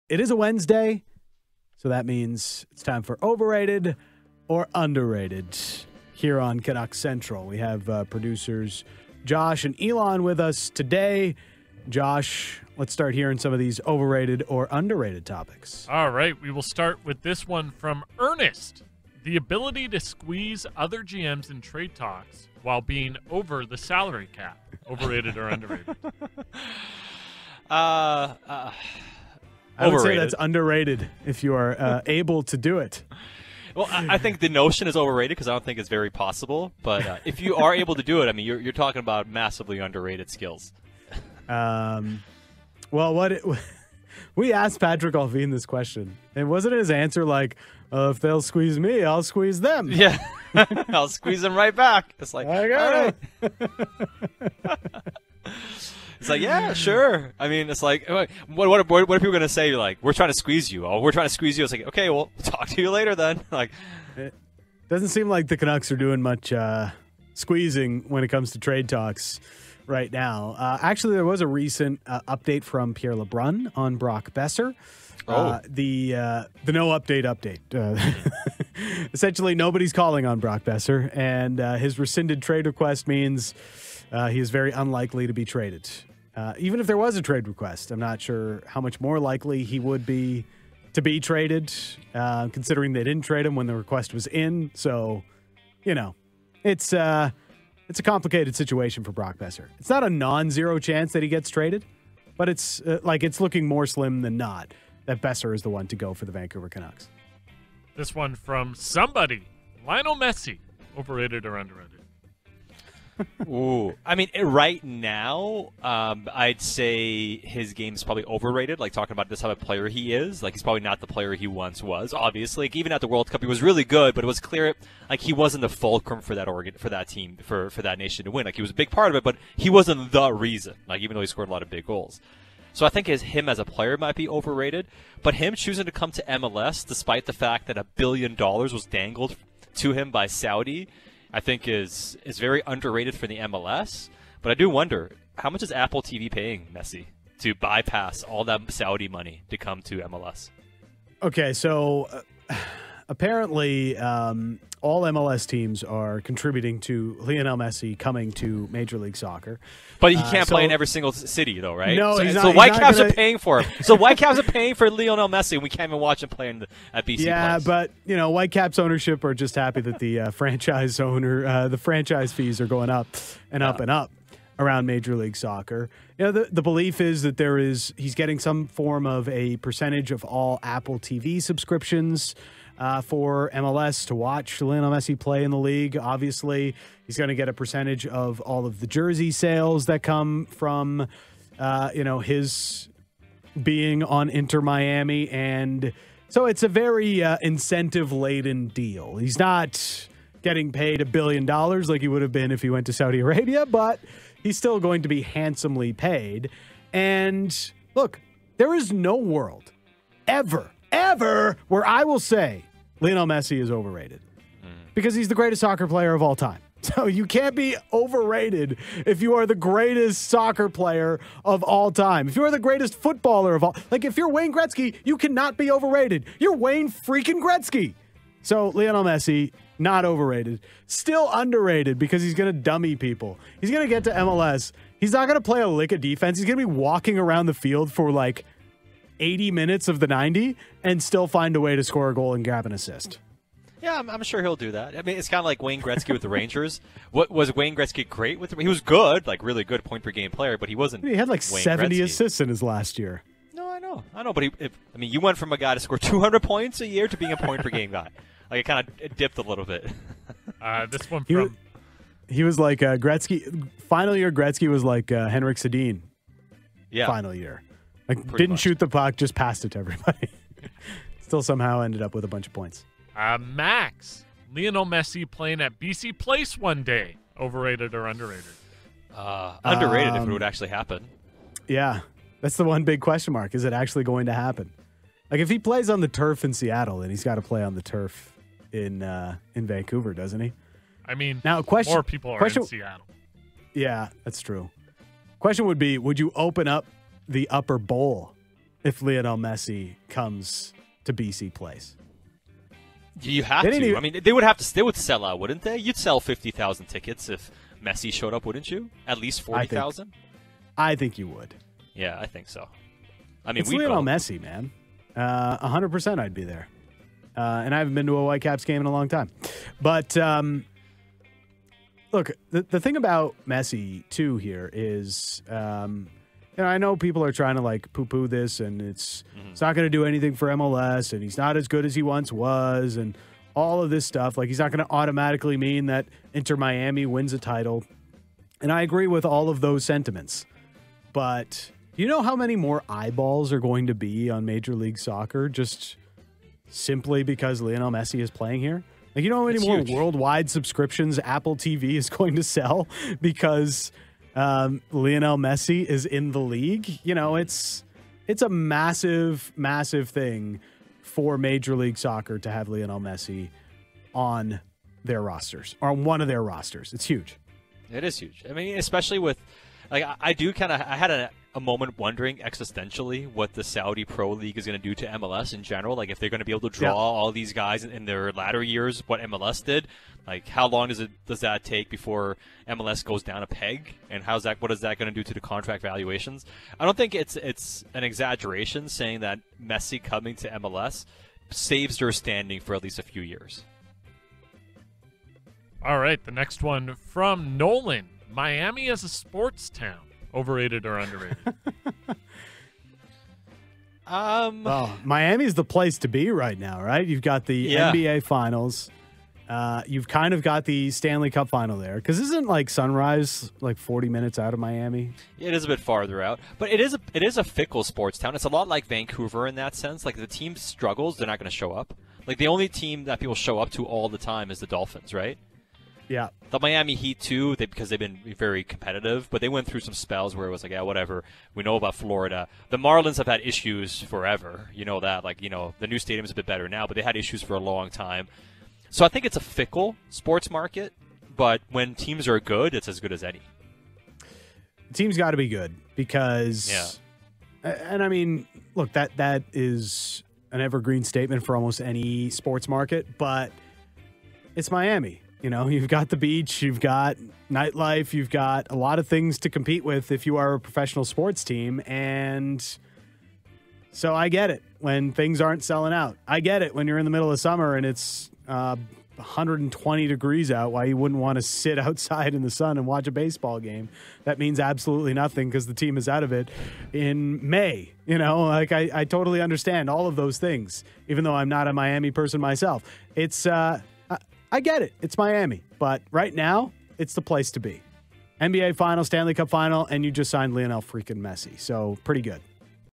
Lionel Messi and Miami as a Sports Town | Canucks Central
It's another edition of Overrated/Underrated as Dan and Sat debate whether topics such as Lionel Messi signing with Miami, Patrik Allvin being able to squeeze other GM's, and much more is overrated or underrated.